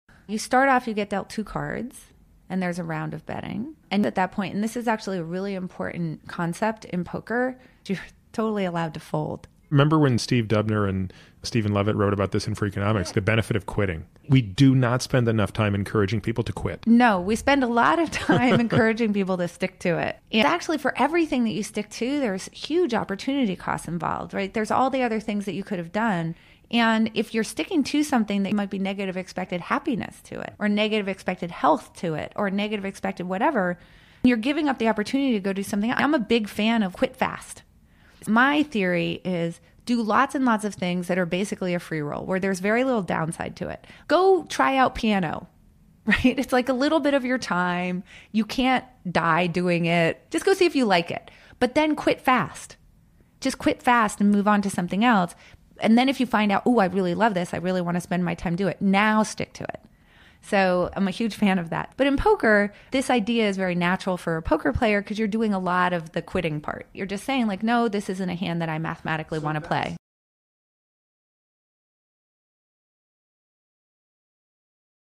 Annie Duke (professional poker player) thinks it's better to quit
"Our culture's telling us to 'stick to it.' I think we should quit fast." – Annie Duke

Annie Duke has a great take on quitting in her interview with Peter Attia.

Transcript:

Annie Duke: You start off you get dealt two cards, and there's a round of betting. And at that point, and this is actually a really important concept in poker, you're totally allowed to fold.

Peter Attia: I remember when Steve Dubner and Steven Levitt wrote about this in Freakonomics: the benefit of quitting. We do not spend enough time encouraging people to quit!

Annie Duke: No, we spend a lot of time encouraging people to stick to it.  Actually, for everything that you stick to there's huge opportunity costs involved, right? There's all the other things that you could have done.

And if you're sticking to something that might have negative expected happiness to it or negative expected health to it or negative expected whatever, you're giving up the opportunity to go do something else. I'm a big fan of "quit fast."

My theory is: do lots and lots of things that are basically a "free roll," where there's very little downside to it. Go try out piano! It's like a little bit of your time. You can't die doing it. Just go see if you like it, but then quit fast! Just quit fast and move on to something else. And then, if you find out "ooh, I really love this, I really want to spend my time on it," now stick to it.

I'm a huge fan of that. But in poker, this idea is very natural for a poker player because you're doing a lot of the quitting part. You're just saying like no this isn't a hand that I mathematically want to play.